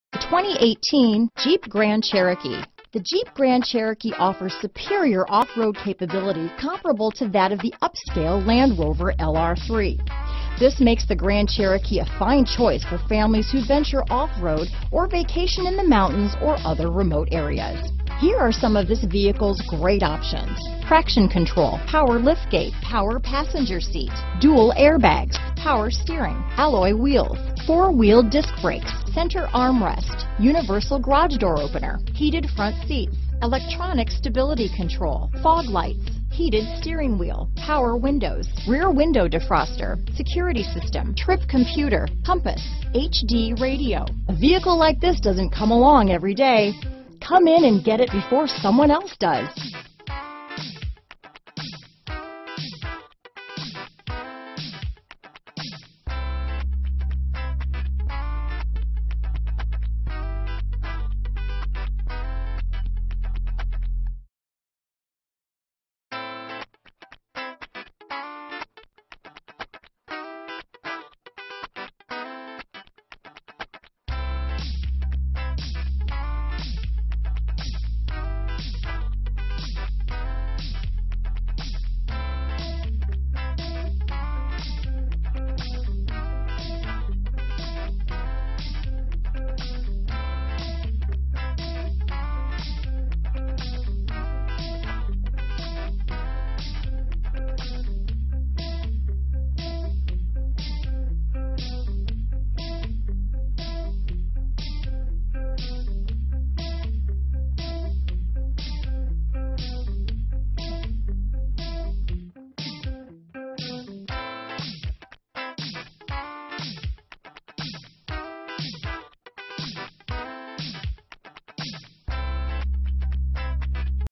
2018 Jeep Grand Cherokee Owosso, East Lansing, Haslett, DeWitt, St. Johns, OK N8330
N 2018 Jeep Grand Cherokee available at Slingerland Chrysler in Owosso, Michigan. Servicing the East Lansing, Haslett, DeWitt, St. Johns, MI area.

Slingerland Chrysler
3640 E M 21

2018 Jeep Grand Cherokee Limited 4WD.